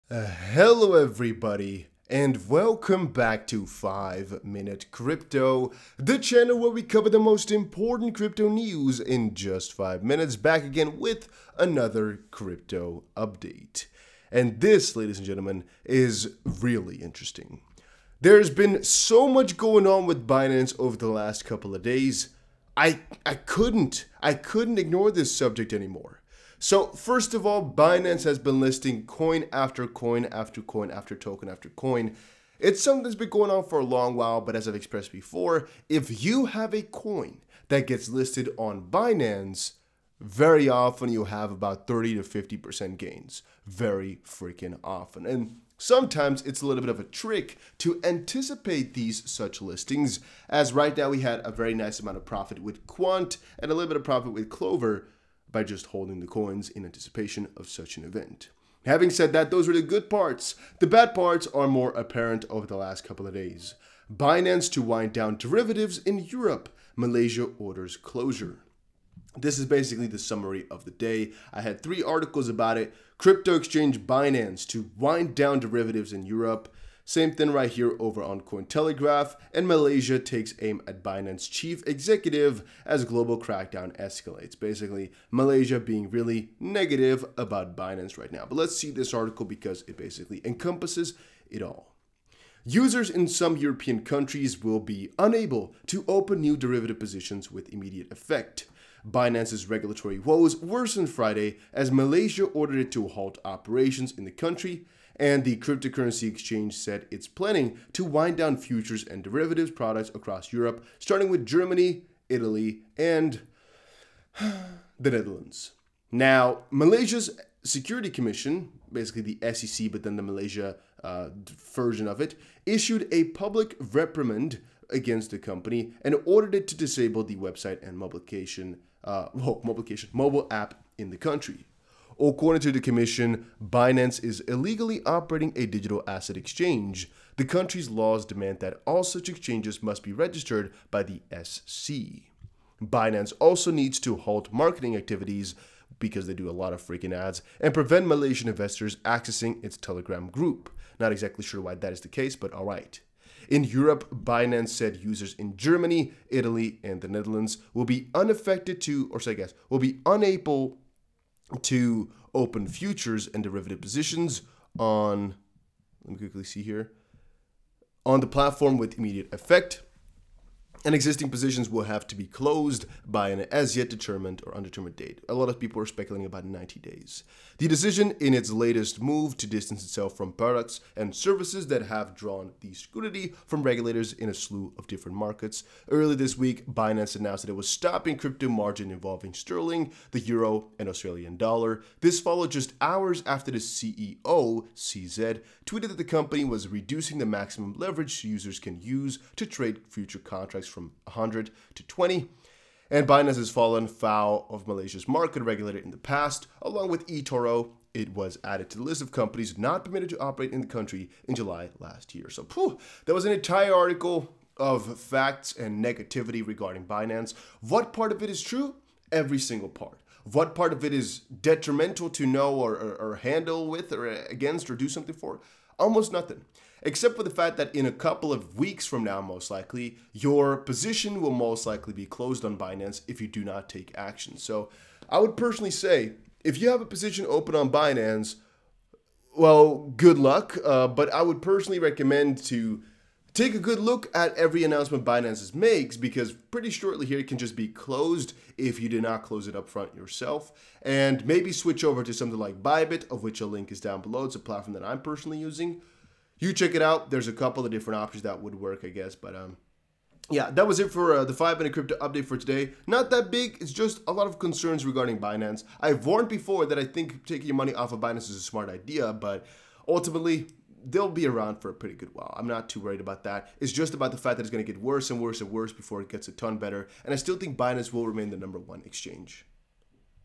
BREAKING: BINANCE JUST GOT BANNED IN MALAYSIA & STOPPED FUTURES TRADING IN EUROPE!!!!!!!!!!!!!!!!!!!
$600 FREE BONUS!!!

These links may earn me some money!